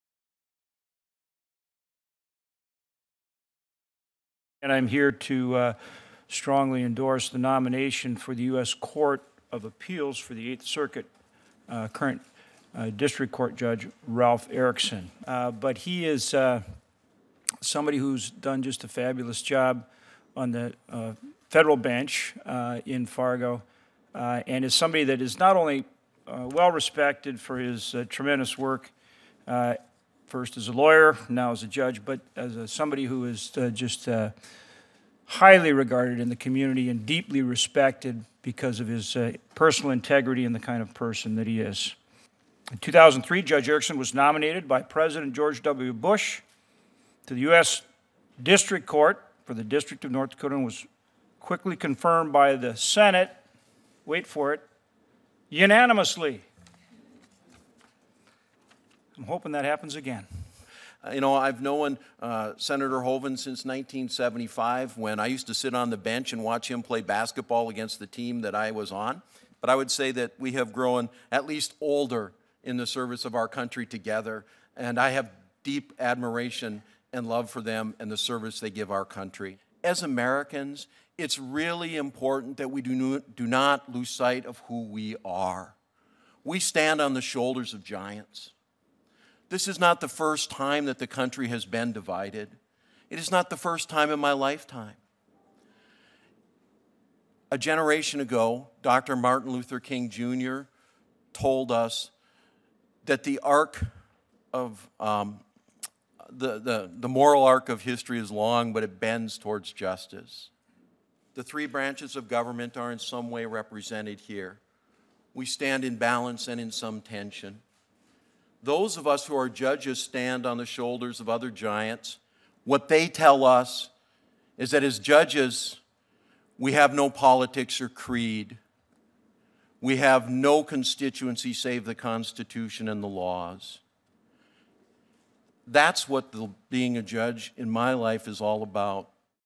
Senator Hoeven and Judge Ralph Erickson at Judiciary Committee Hearing
Yesterday I introduced U.S. District Judge Ralph Erickson, who has been nominated to serve on the 8th Circuit Court of Appeals, at a Senate Judiciary Committee hearing. A longtime North Dakotan, Judge Erickson has demonstrated a deep respect for the Constitution and the rule of law. He is incredibly qualified to serve on the 8th Circuit Court of Appeals and I am confident he will be an excellent addition to the 2nd highest court in the United States.